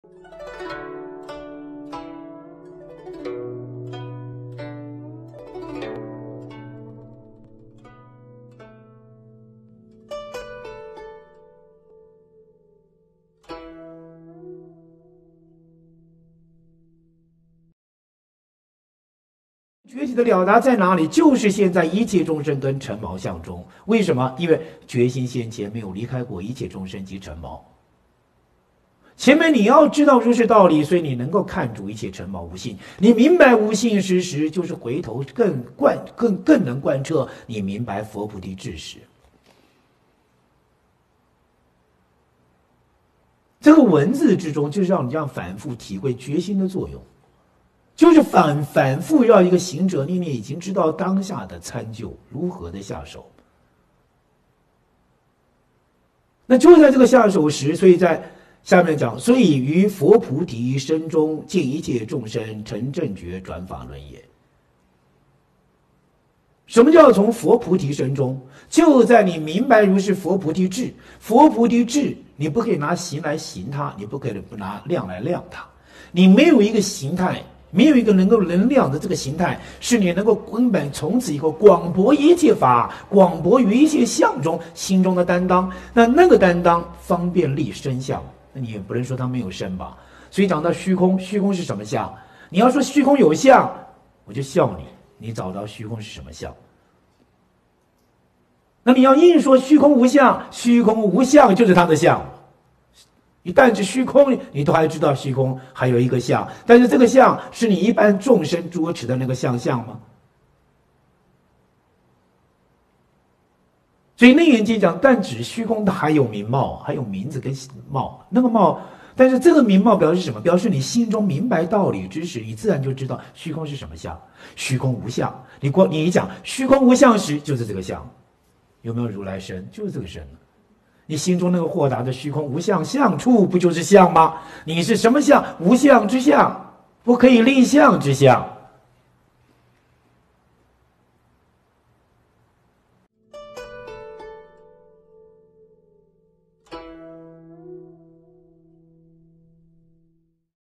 覺體的了達在哪裡？就是現在一切眾生跟塵毛相中—2022華嚴經義海百門節錄-自然和尚宣講
華嚴經義海百門節錄-自然和尚宣講7-3

【覺體的了達在哪裡？就是現在一切眾生跟塵毛相中】

覺體的了達在哪裡？

就是現在一切眾生跟塵毛相中。

為什麼？

因為覺心現前沒有離開過一切眾生及塵毛。

前面你要知道如是道理，所以你能看著一切塵毛無性。

你明白無性之時，就是回頭更能貫徹你明白佛菩提智時，這個文字之中就是讓你這樣反復體會覺心的作用，就是反復要一個行者念念已經知道當下的參究，如何的下手嘛。

那就在這個下手時，所以在下面講「所以與佛菩提身中，見一切眾生成正覺，轉法輪也」。

什麼叫從佛菩提身中？就在你明白如是佛菩提智，佛菩提智你不可以拿形來形他，你不可以拿量來量他，你沒有一個形態。沒有一個能量的這個形態，是你能夠根本從此以後廣博一切法，廣博於一切相中心中的擔當。

但那個擔當，方便立身相。那你也不能說他沒有身吧？所以講到虛空，虛空是什麼相？你要說虛空有相，我就笑你。你找到虛空是什麼相？那你硬要說虛空無相，虛空無相就是他的相嘛。

你但指虛空，你都還要知道虛空還有一個相。

但是這個相是你一般眾生捉持的那個相相嗎？所以《楞嚴經》講但指虛空，他還有名貌啊，還有名字跟貌，那個貌。

但是這個名貌表示什麼？表示你心中明白道理之時，你自然就知道虛空什麼相，虛空無相
你講，虛空無相時，就是這個相啊。

有沒有如來身？就是這個身吶。

你心中那個豁達的虛空無相相處，不就是相嗎。

你是什麼相？

無相之相，不可以立相之相。

自然法師 | 覺體的了達在哪裡？就是現在一切眾生跟塵毛相中。

《華嚴經義海百門》39-1集節錄